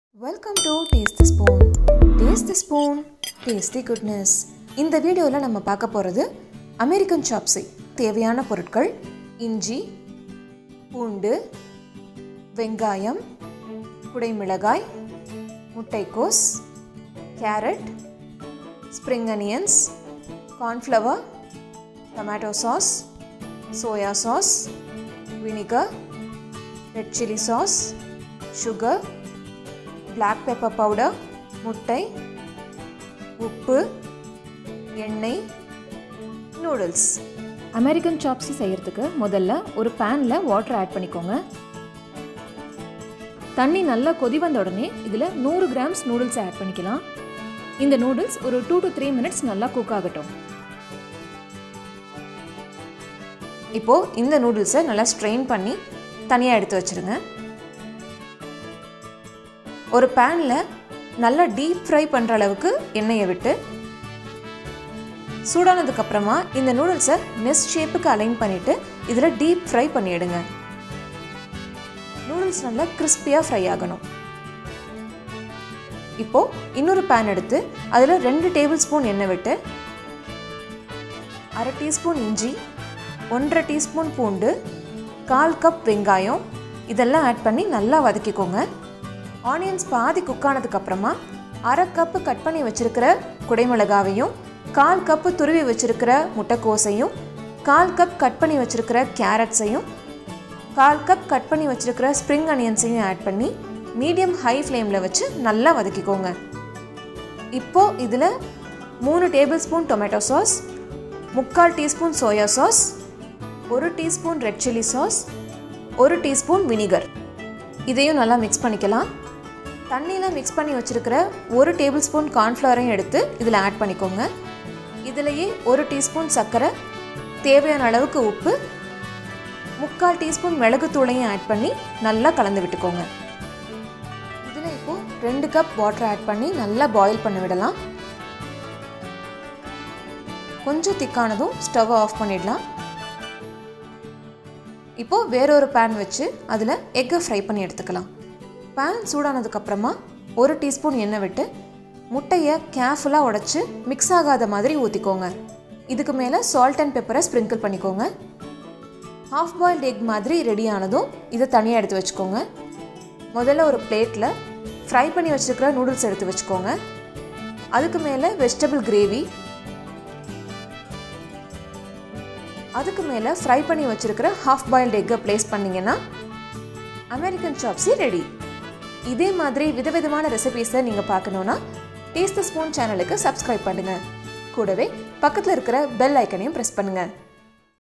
American Chop Suey TasteTSpoon - Cooking Show
American Chop Suey

The chopsuey recipe is invented by the American Chinese as an amalgamation to Chinese and American
cuisine. Basically, the Chinese community settled in America started to make their own gravy and served
it with deep-fried noodles. Today it is popularly known as American chop suey recipe which can be made
with myriad varieties.

Ingredients

 Noodles 100 g
 Vegetable Oil  2½ tsp
 Ginger (Finely Chopped) ½ tsp
 Garlic (Finely Chopped) 1½ tsp
 Onion (Sliced) ¼ cup
 Mixed peppers (Julienned) ½ cup
 Cabbage (Shredded) ¼ cup
 Carrot (Julienned) ¼ cup
 Spring Onion Greens  ¼ cup

Method

Boil the noodles, Strain and keep aside. Heat oil in a pan and fry noodles in. When frying the
noodles, carefully place the noodles in such a way to form a nest. Deep fry the noodles till crisp from
both sides Let it cool down.
Meanwhile, heat 2 tbsp. oil in a wok add ginger and garlic and fry them for just 30 secs on high
flame, add onions and sauté them till translucent.
Add bell peppers, cabbage and carrots and cook them on medium high flame for 3-4 mins add
spring onion greens and Mix well.
Mix cornflour in ⅛th cup of water and mix well and keep it aside
Add all the sauces to the wok add more tomato sauce if desired as American chop Suey is little
tangy and sweet now add sugar, salt and pepper powder mix well.
Add 2 cups of water to the wok and bring it to a boil, add the corn flour water mixture and let the
gravy become little bit thick turn off the flame.
Now make fried egg, heat and grease a griddle pan with oil carefully break an egg without mixing
yolk and white like we do for bull’s eye sprinkle some salt and pepper on it. When the egg cooked
remove from the pan

Now assemble the dish, first place the noodles nest without breaking on a plate pour the gravy with
veggies over it finally place a fried egg on it serve immediately.